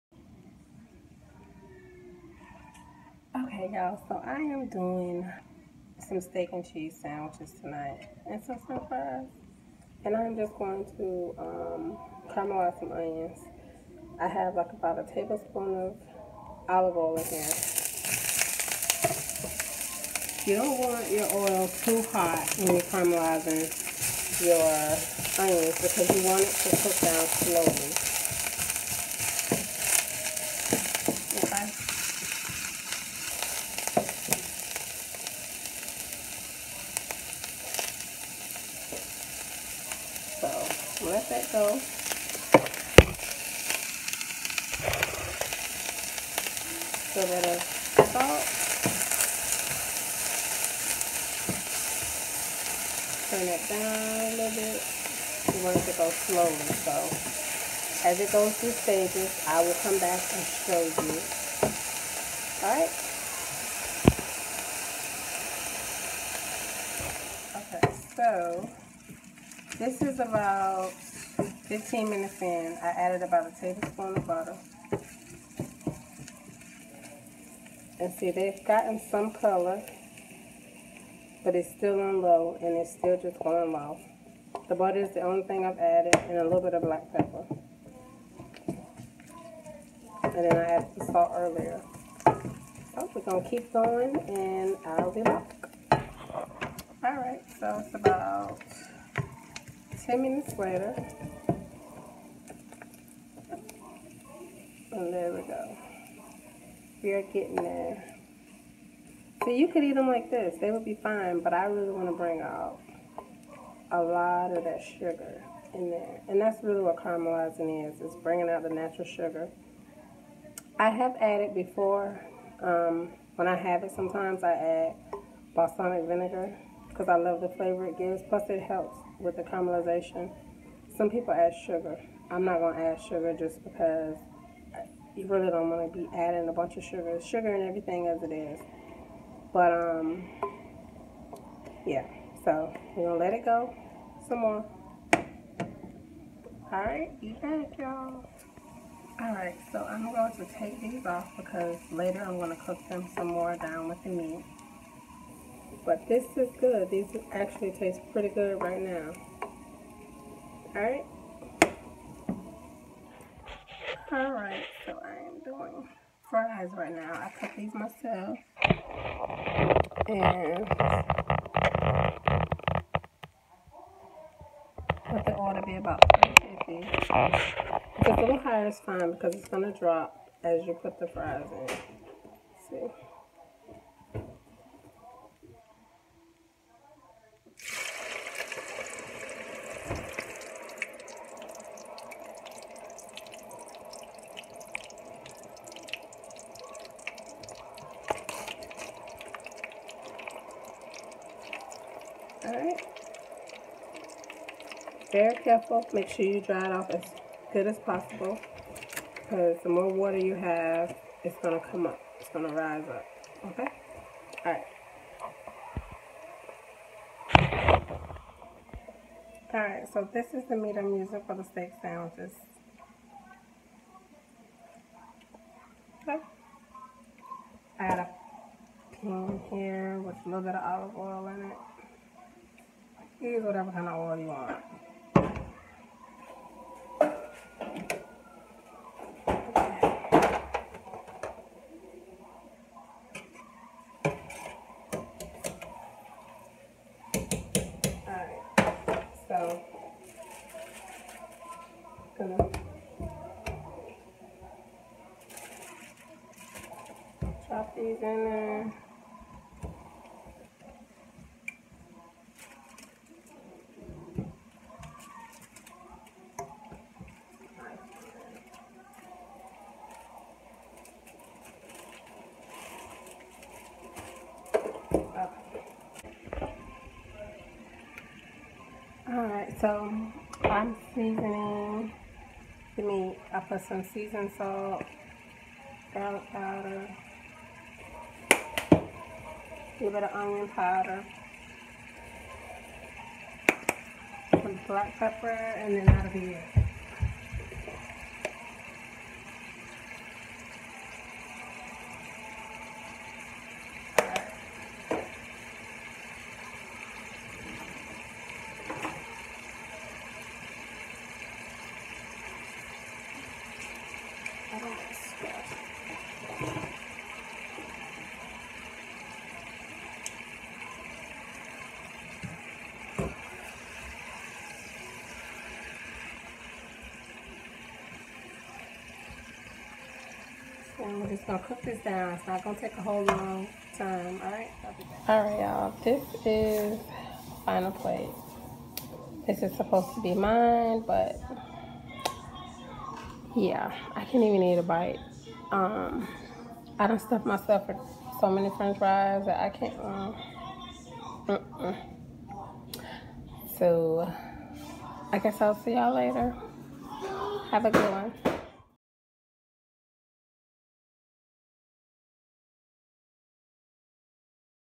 Steak N Cheese Sub
I usually like thinly sliced ribeye but this was more about a quick meal. It was good! Everyone ate theirs so it was definitely a winner! If there's anything you'd like to see on this channel let me know!